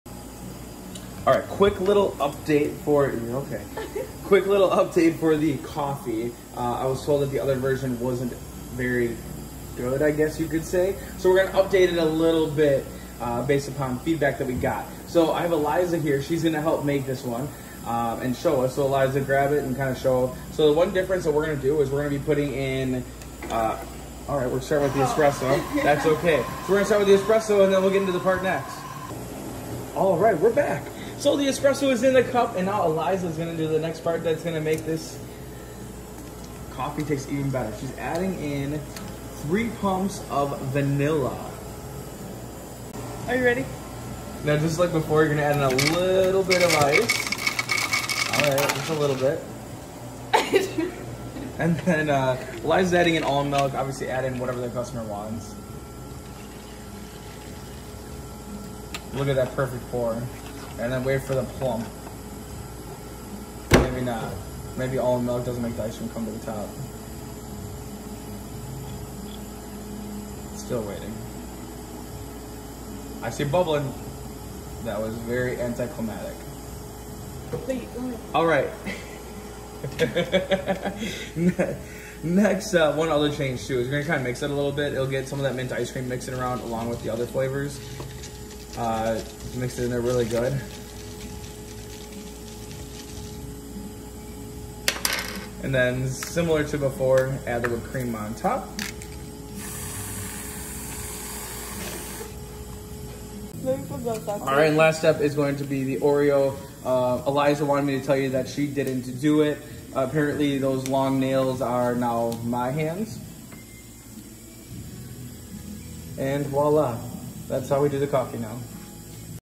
Updated Grasshopper Coffee
We updated the Grasshopper Coffee, watch to see what we changed.